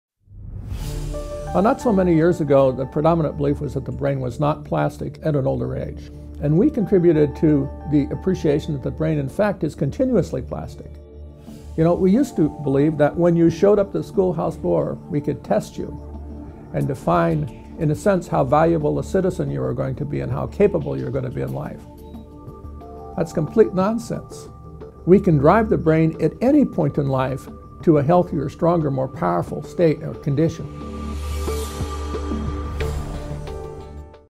Michael Merzenich Rethinks How Brains Can Change
Michael Merzenich demonstrated that sensory circuits in the cerebral cortex can be reorganized by experience in adulthood, suggesting ways to maximize learning and recovery after brain injury.